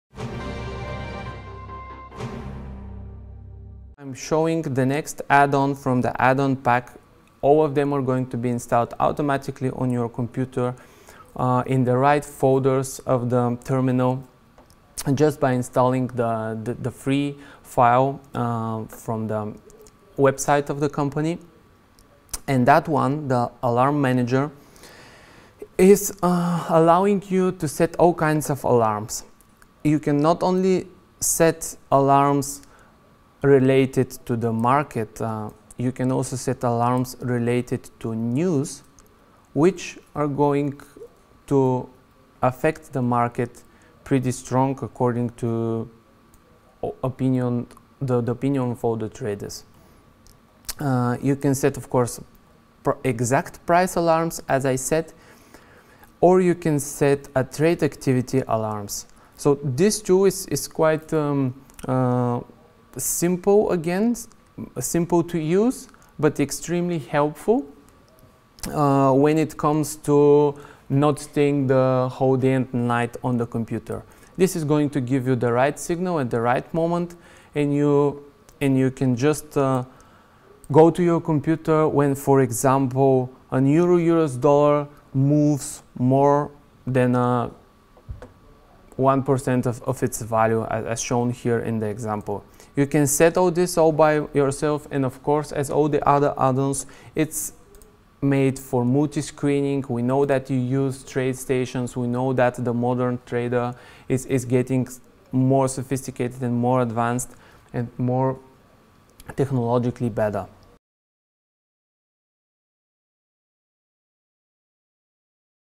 MT4+ Alarm Manager
Get notifications about events on your account and in the market, take automatic actions when such events occur, and send automatic updates to your followers via Twitter, email or SMS. The Alarm Manager helps you focus on the creative and/or analytical side of trading.